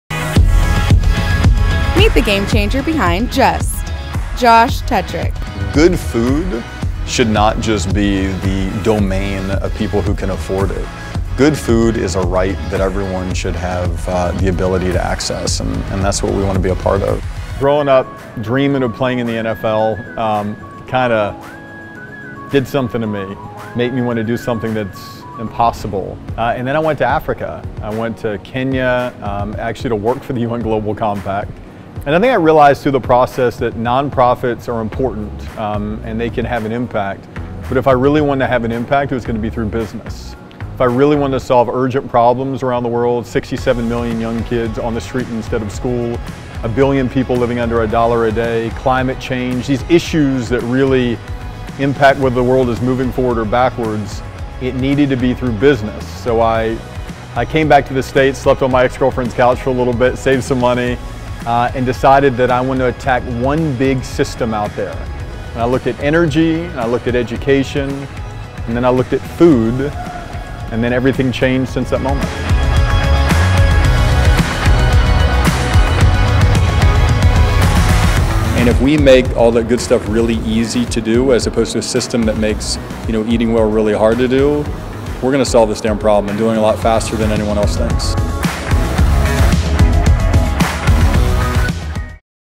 Game Changers: Josh Tetrick
Meet the incredible CEO behind the revolutionary JUST egg